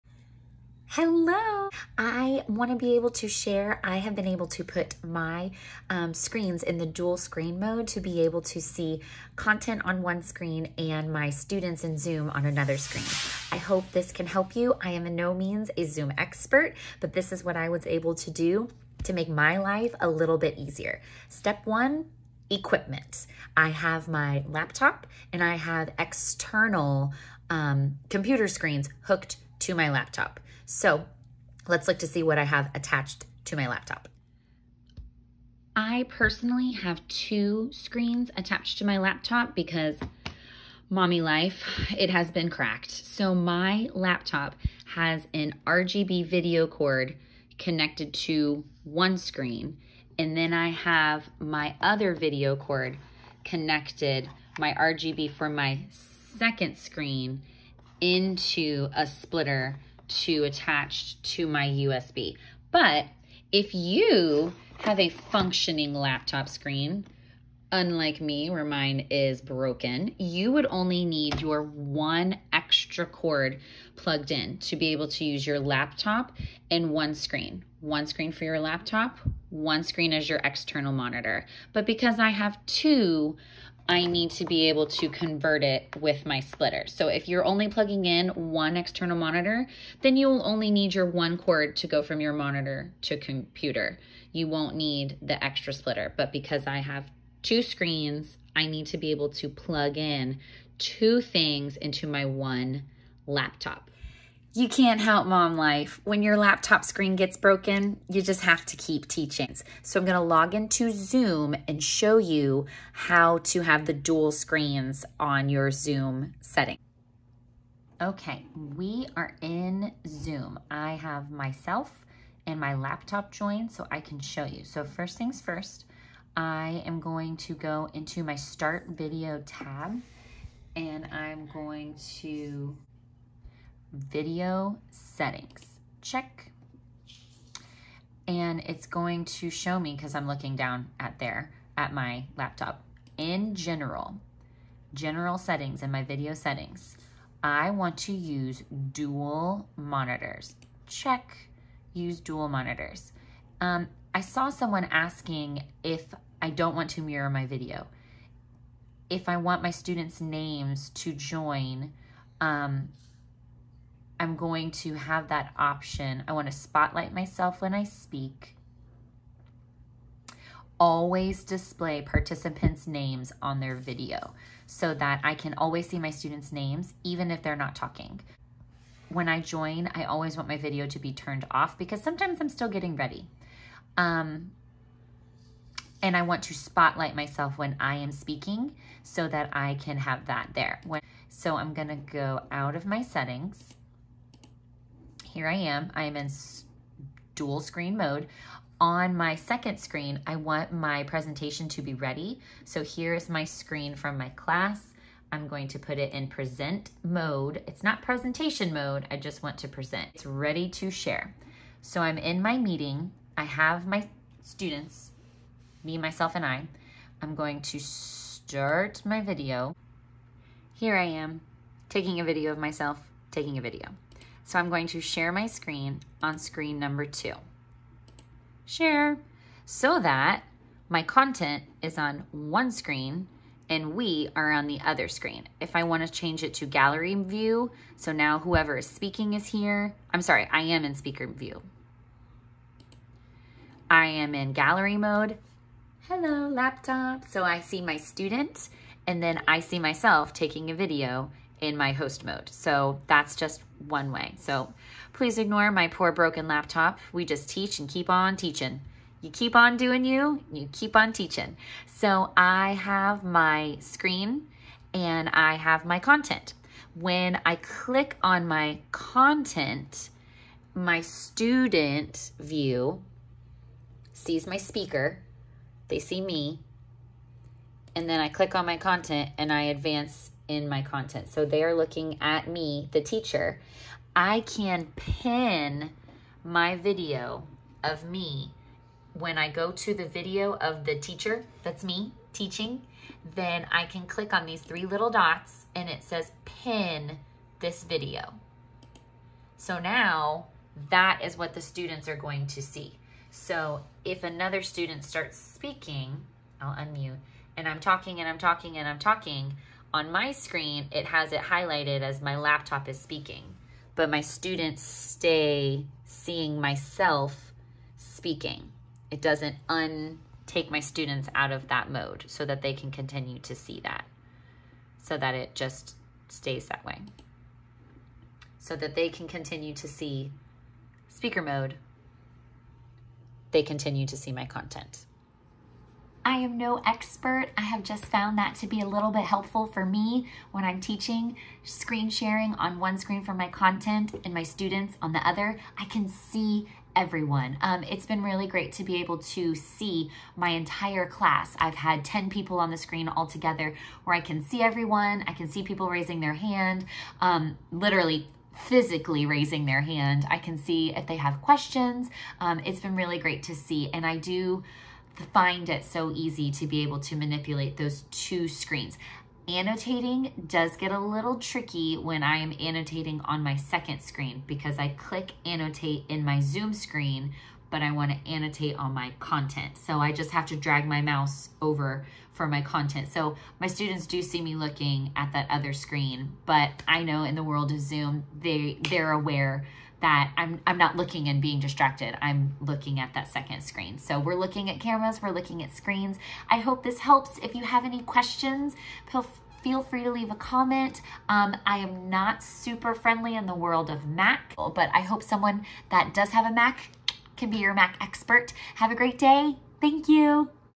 Dual Screens on Zoom to Teach
What I have learned about dual screen mode on zoom. How to share content and see students. Pin a video on zoom and a few of the setting features.